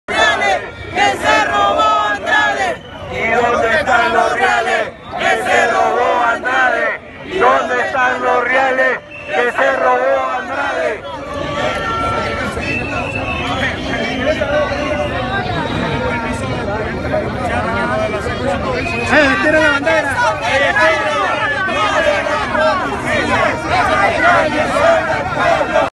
"¿Dónde están los reales que se robó Andrade?": Trabajadores exigen mejoras de salarios
"¿Dónde están los reales que se robó Andrade?", corean sindicalistas de diversos sectores del país, quienes exigen mejoras salariales y se dirigen al Ministerio Público